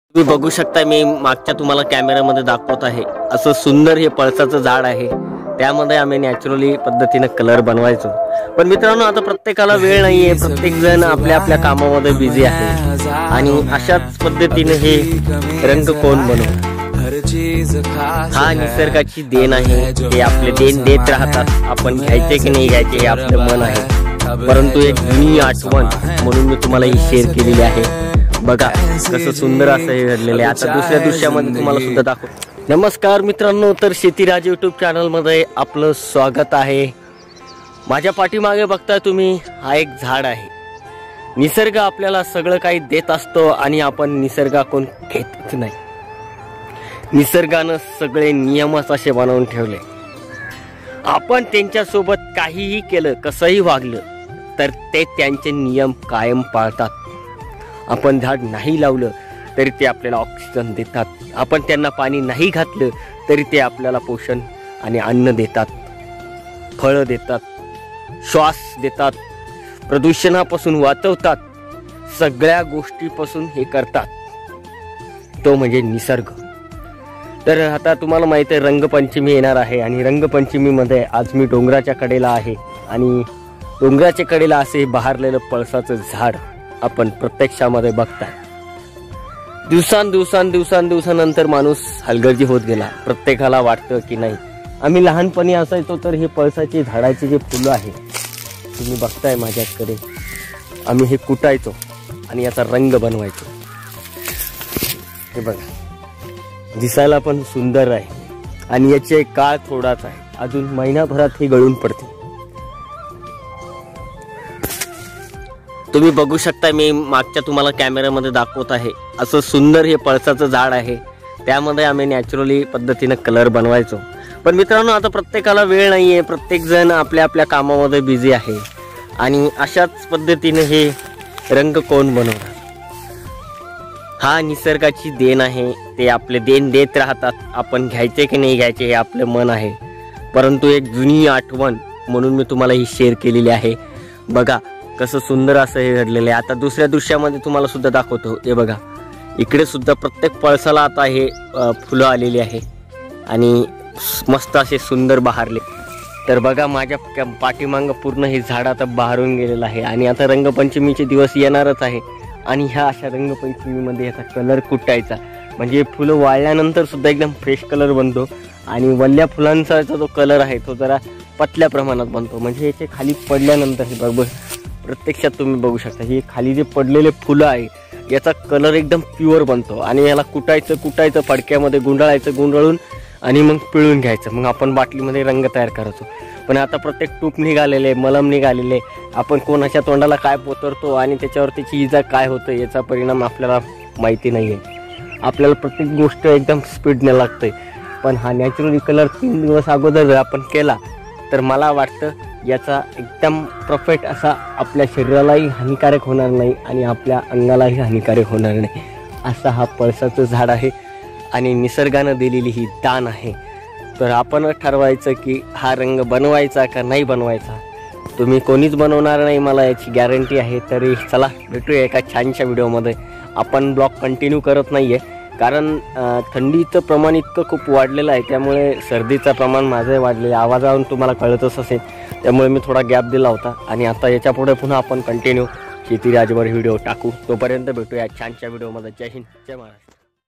आपली एक जुनी आठवण या/ फुलापासून आपण रंग बनवायचं रंगपंचमीसाठी रंगपंचमीसाठी नॅचरल पद्धतीने कलर कसा
I नमस्कार मित्रांनो आजच्या ब्लॉगमध्ये आपण दाखवणार आहे की जंगलामध्ये हा असा पळसाचे झाड आहे की त्या पावसाच्या झाडावर आपण फुल असायचे ते फुल आपण कुठे आहे तो आणि नॅचरली पद्धतीने कलर बनवायचं तर आजचा हा ब्लॉग थोडक्यामध्ये तुम्हाला सांगितलेला आहे कशाप्रकारे रंगपंचमीसाठी फुल करून आपण हा रंग बनवायचं धन्यवाद आवडला तर लाईक सबस्क्राईब नक्की करा